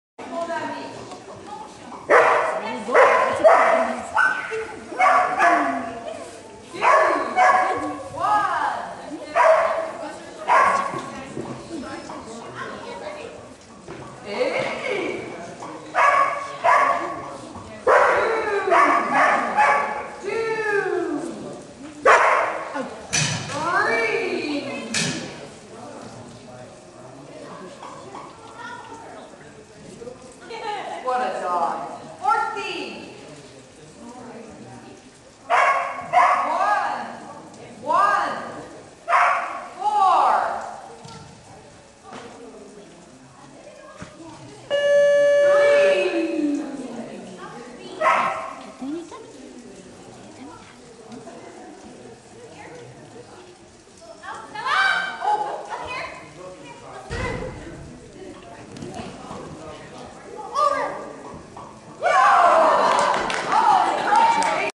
Havanese Master Gamble DAWG Nanaimo AAC
Bella manages to Q this master gamble!  Squeaked under the buzzer!